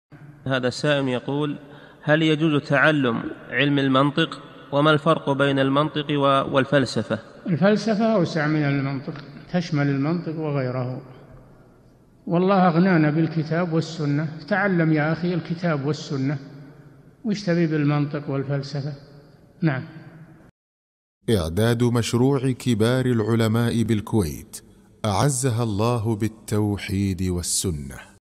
[419 -560] هل يجوز تعلم علم المنطق، وما الفرق بين المنطق والفلسفة؟ - الشيخ صالح الفوزان
-"السؤال: هل يجوز تعلم علم المنطق، وما الفرق بين المنطق والفلسفة؟
الجواب: الفلسفة أوسع من المنطق تشمل المنطق وغيره، والله أغنانا بالكتاب والسنة، تعلم يا أخي الكتاب والسنة، ما تريد بالمنطق والفلسفة؟
للاشتراك في قناة الشيخ صالح الفوزان
سؤال وجواب من شرح السنة للبربهاري ( 560 ) مقطع

للاستفسار والإقتراحات والدعم
رئيس الفريق
لمشروع كبار العلماء